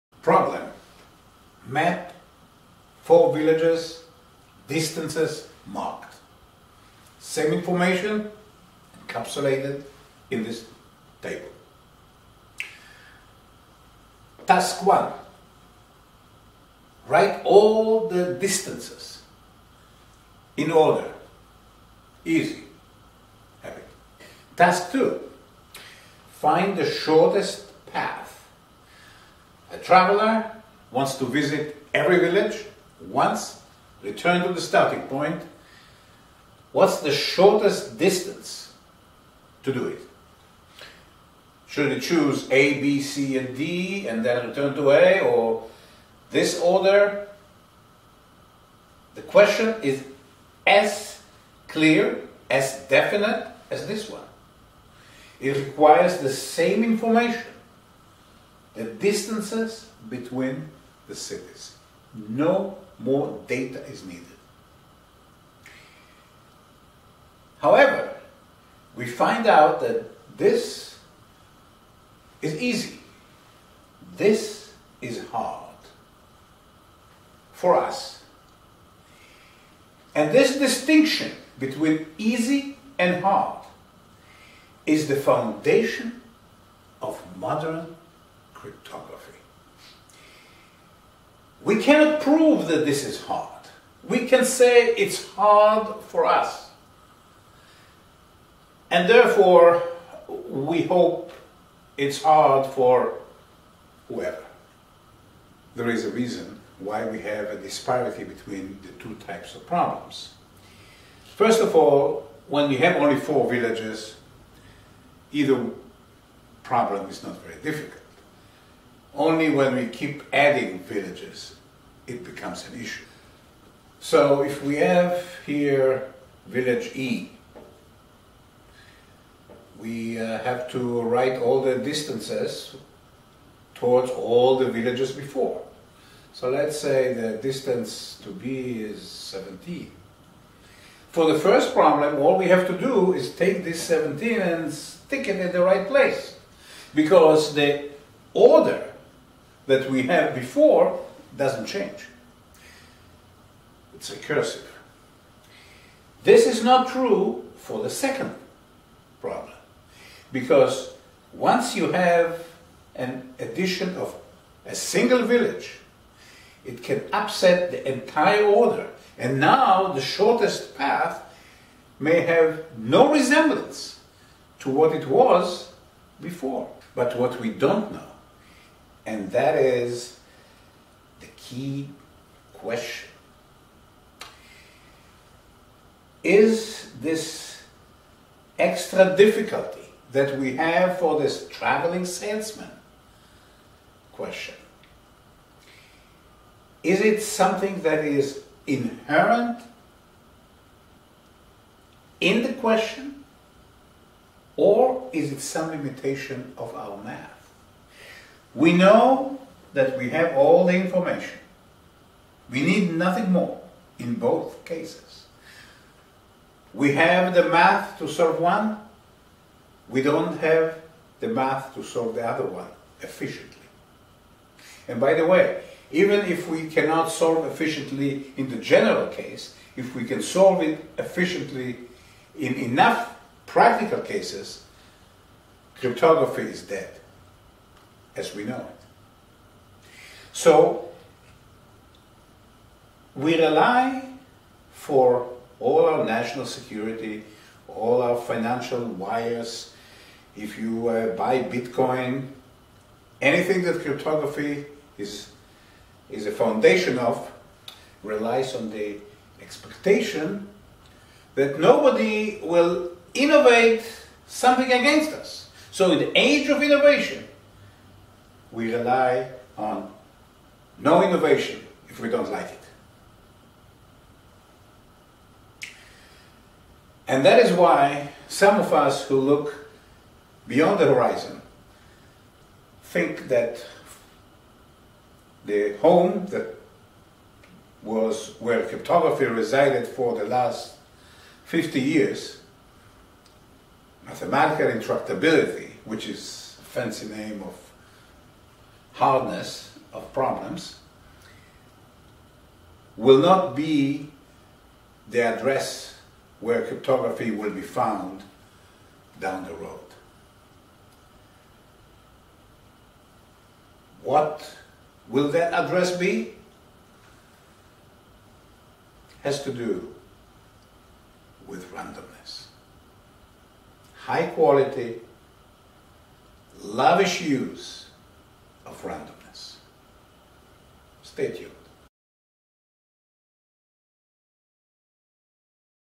The Shaky Essence of Modern Cryptography
Modern cryptography hinges on the existence of solvable mathematical puzzles which we hope others are too dumb to solve efficiently. Innovative cryptography is based on the assumption that our attackers lack an innovative spirit. If we (so smart as we are) think a problem is hard, then our adversaries (dumb as they are) will surely be frustrated by it. This is the shaky essence of our cyber security. Unproven mathematical intractability underlies our national security, supports financial transmissions, is foundational in crypto currencies. This video highlights this inconvenient fact, and points to a strategic solution: quantum grade randomness (to be elaborated on in the next lesson).